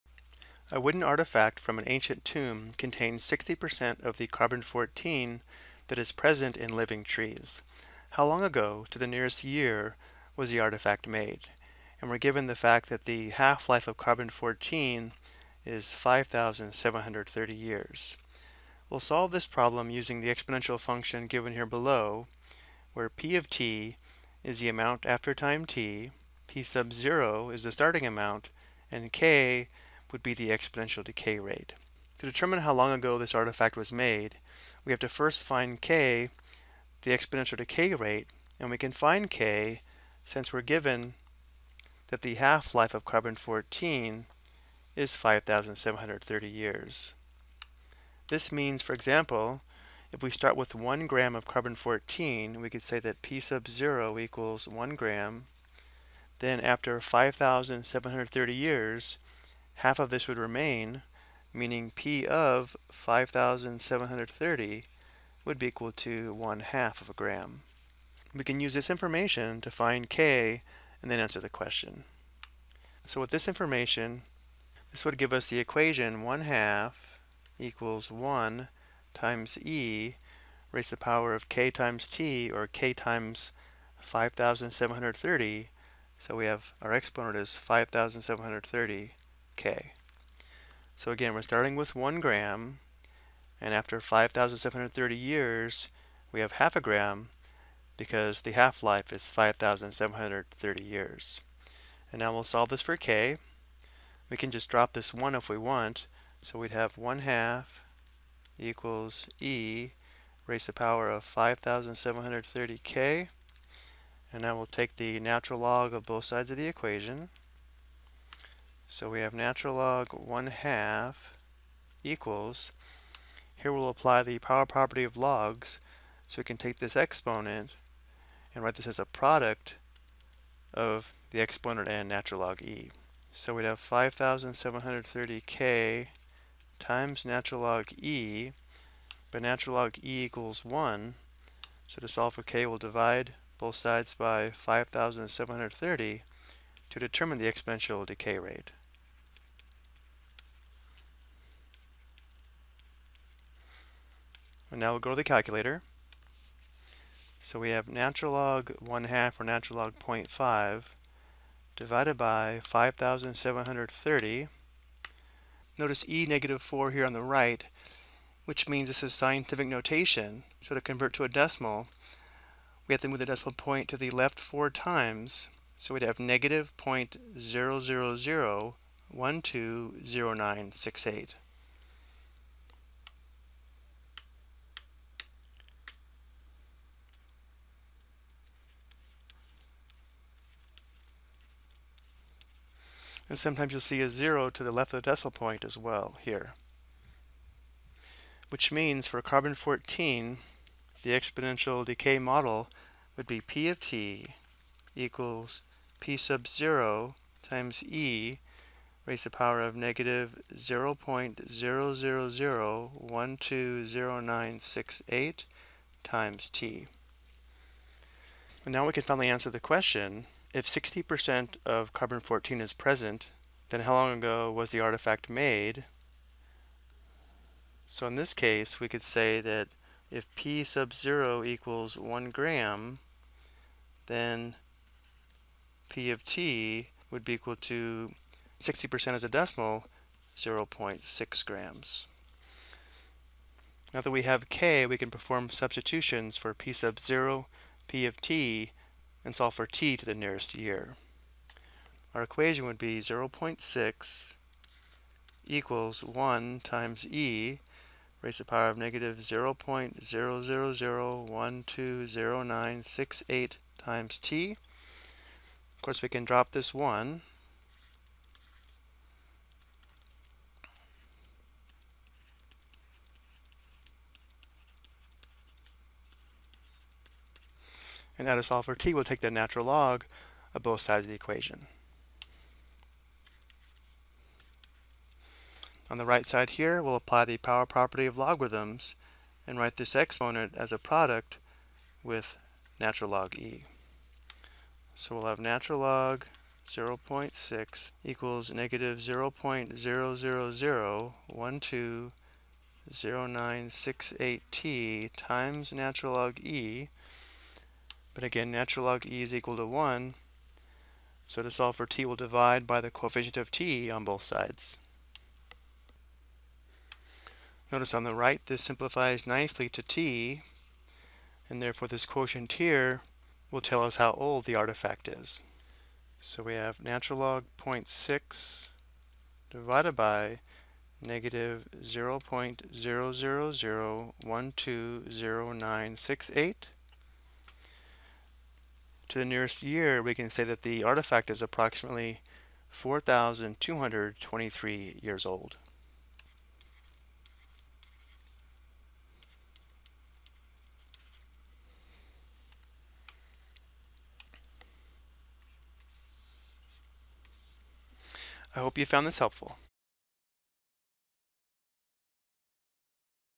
Ex: Exponential Model - Determine Age Using Carbon-14 Given Half Life
This video explains how to use half like to determine an exponential decay rate and then determine how old an artifact is given a percent of carbon-14 remaining.